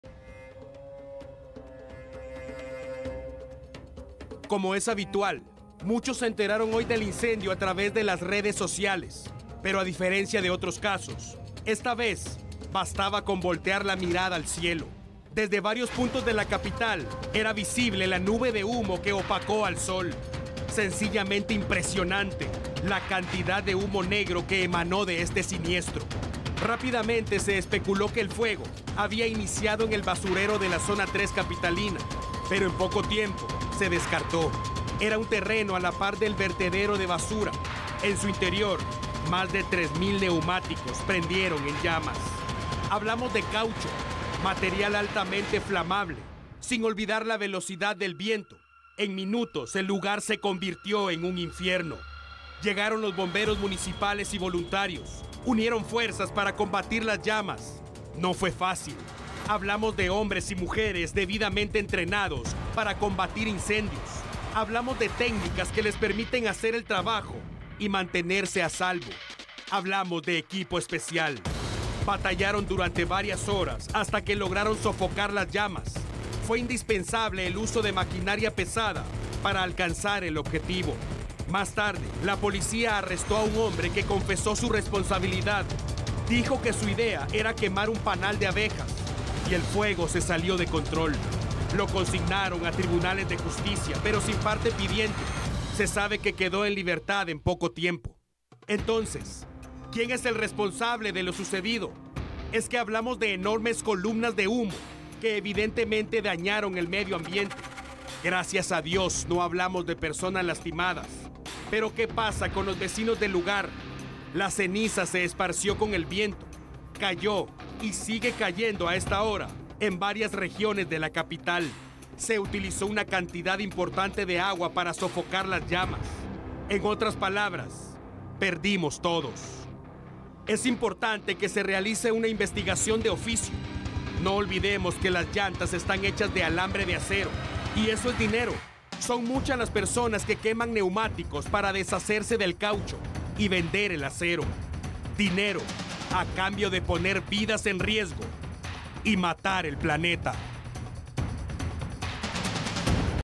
Más de 3 mil neumáticos prendieron en llamas
bomberos Municipales y voluntarios unieron esfuerzos para combatir las llamas

Desde varios puntos de la Capital se hizo visible la nube de humo ocasionada por la quema de neumáticos. Debido a la magnitud del siniestro, bomberos Municipales y voluntarios unieron sus esfuerzos para sofocar las llamas. Sobre este caso, un hombre fue detenido tras haber confesado sus responsabilidad. Dijo que su idea era quemar un panal de abejas y el fuego se salió de control.